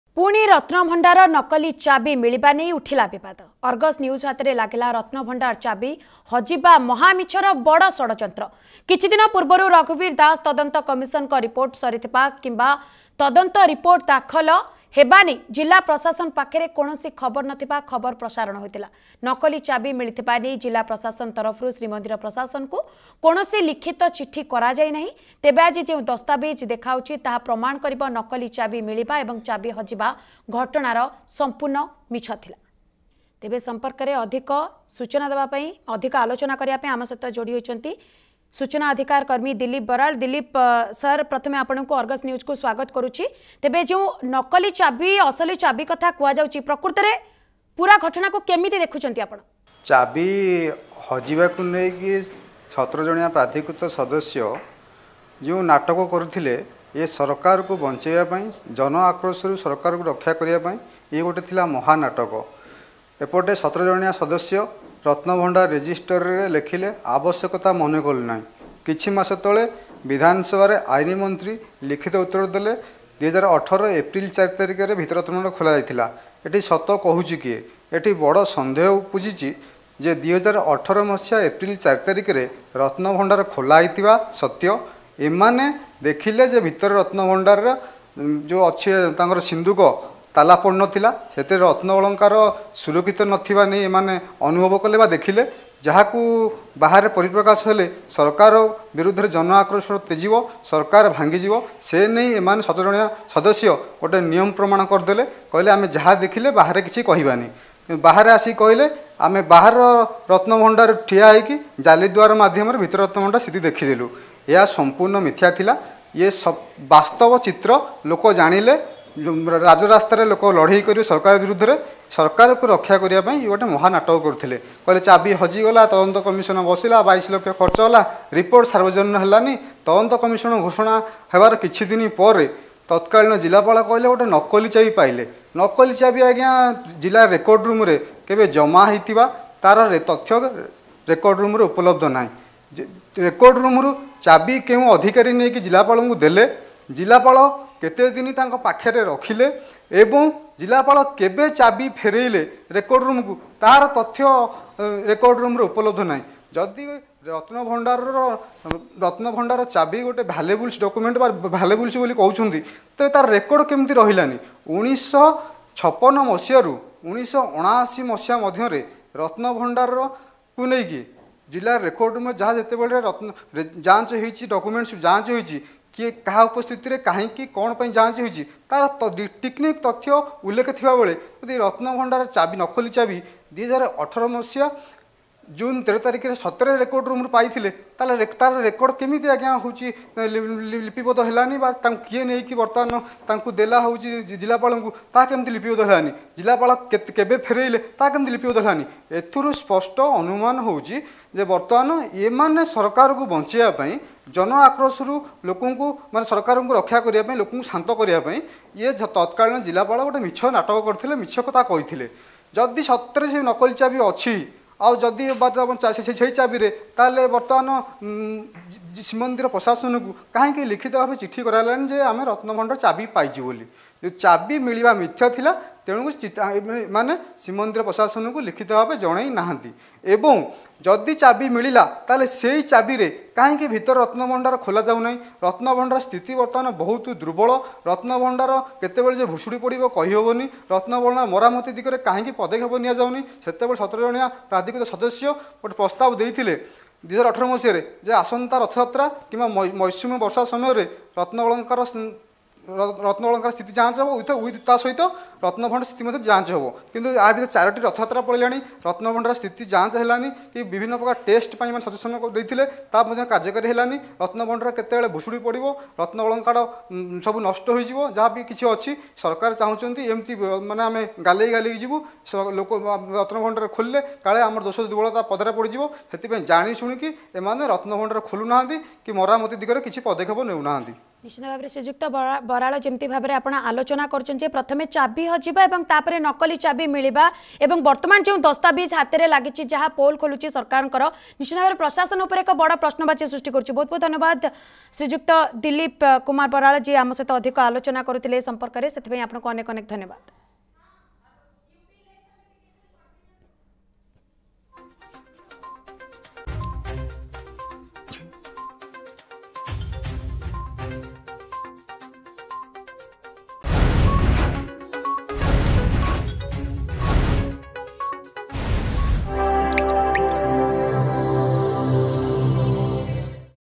Puri: Controversy Over Lord Jagannath's Ratnabhandar Key remerges again
Controversy Over Lord Jagannath's fake Ratnabhandar Key remerges again.